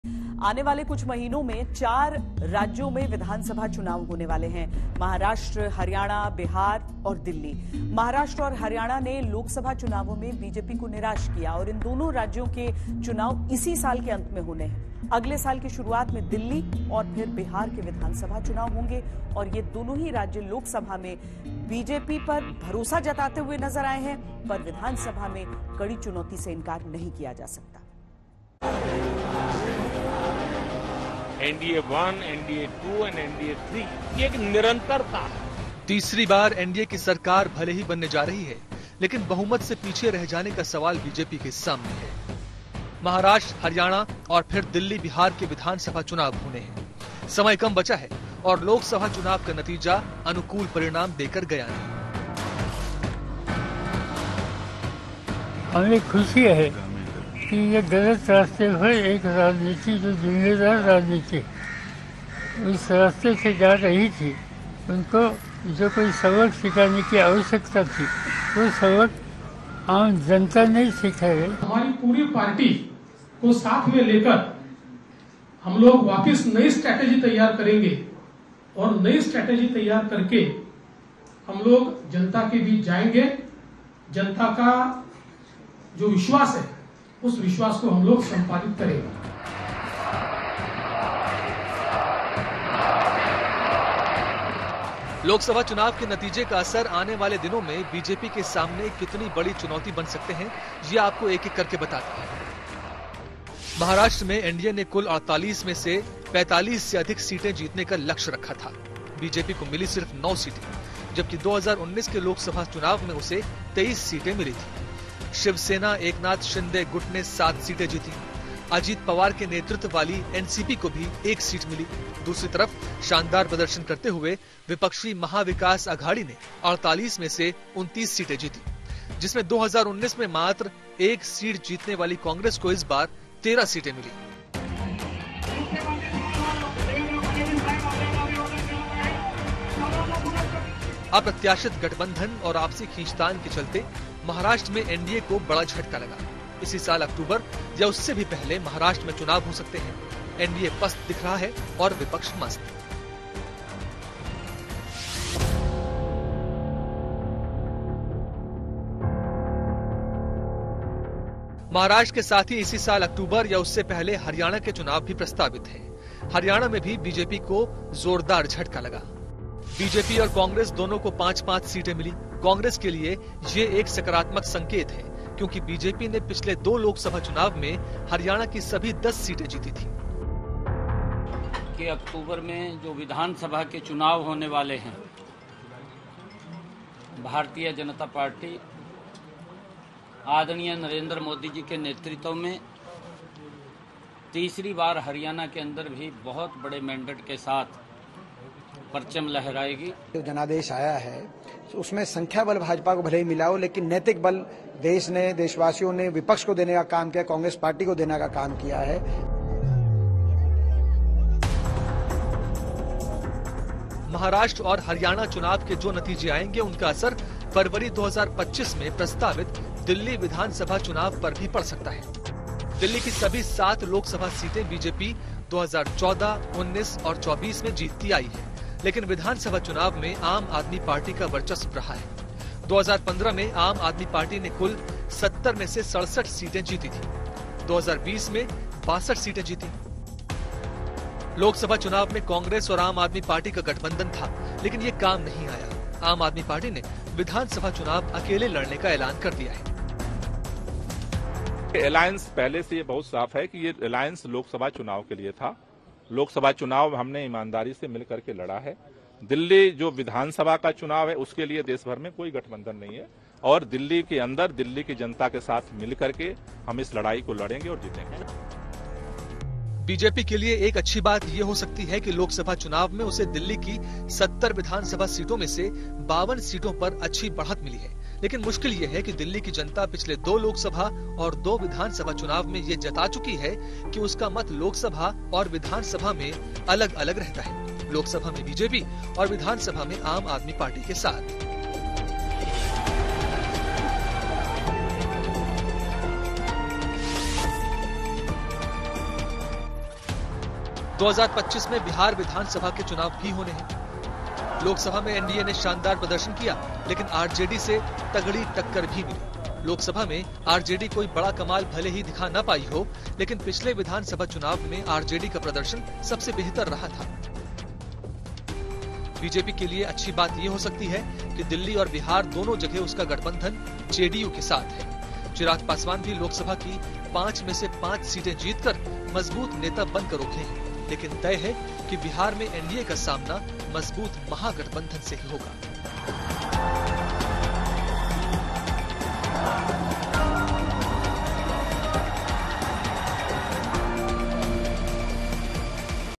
Maharashtra, Haryana, Bihar और Delhi में Vidhan Sabha Elections के लिए कौन-कितना तैयार? | Aaj Tak
आने वाले कुछ महीनों में चार राज्यों में विधानसभा चुनाव होने वाले हैं, महाराष्ट्र, हरियाणा, बिहार और दिल्ली, महाराष्ट्र और हरियाणा में इसी साल विधानसभा चुनाव होने हैं, जबकि दिल्ली और बिहार में अगले साल के शुरुआत में विधानसभा चुनाव होने हैं, महाराष्ट्र और हरियाणा में लोकसभा चुनाव के नतीजों ने बीजेपी को निराश किया है.